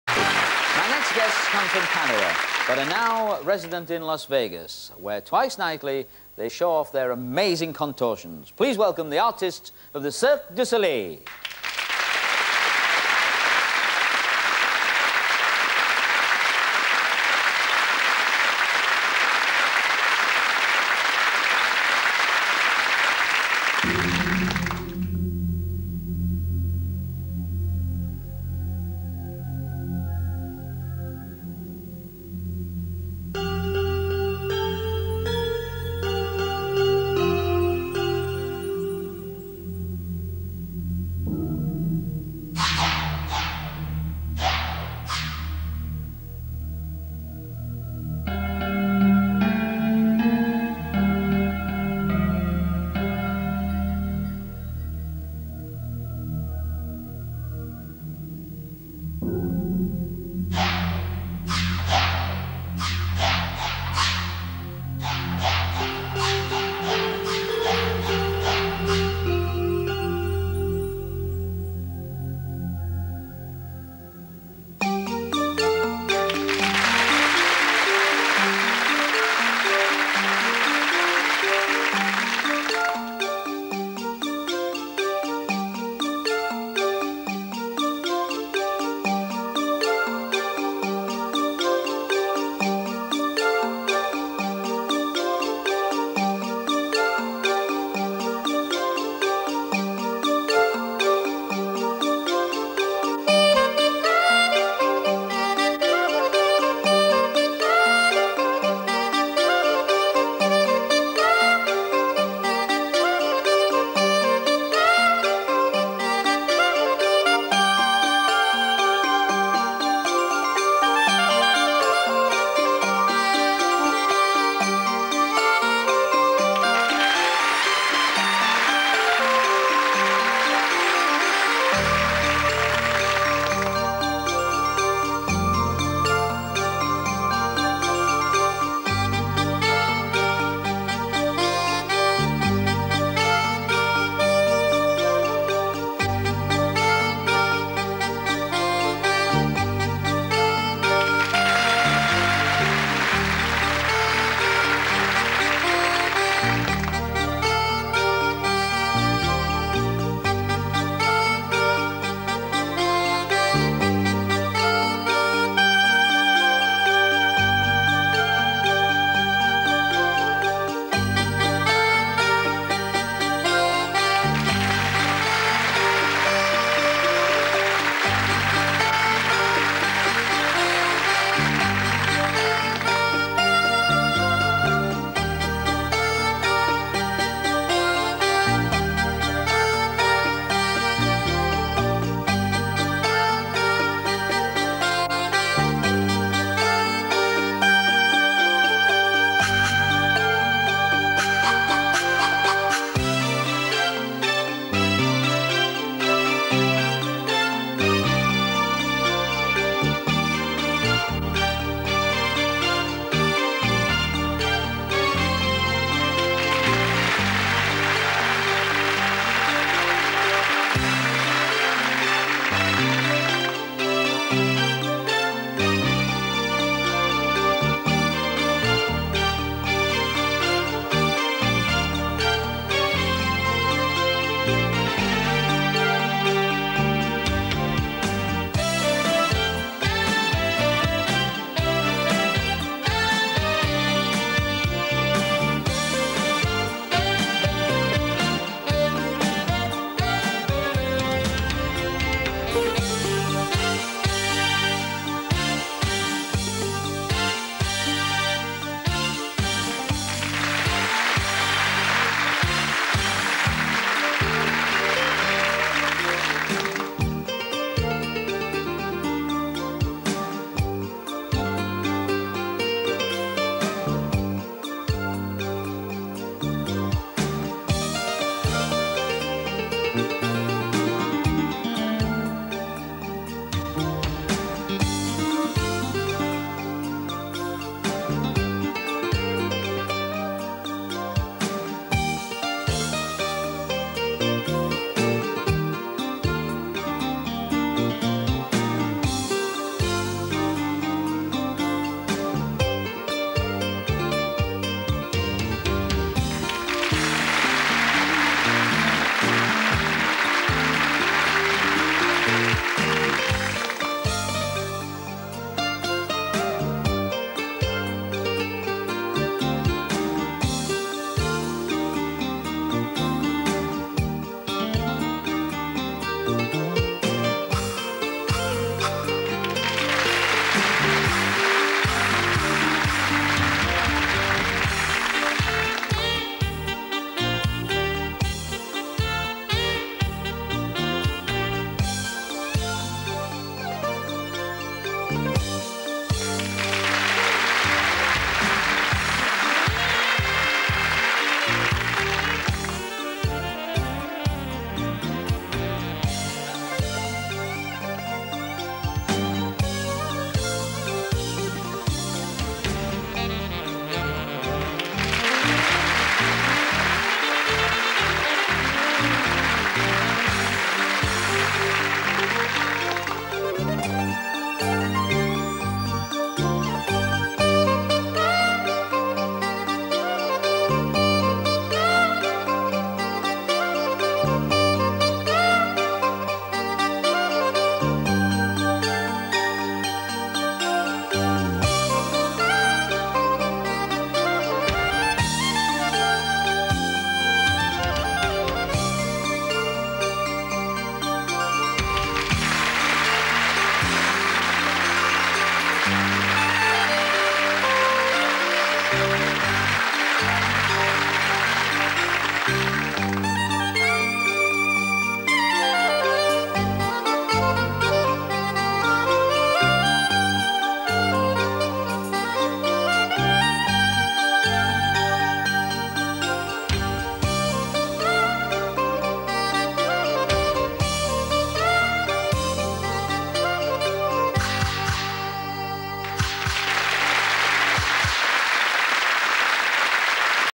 Cirque du Soleil contortionists / групповой каучук, 1994
Jimmy Jacinto, Nadine Binette, Isabelle Chasse and Laurence Racine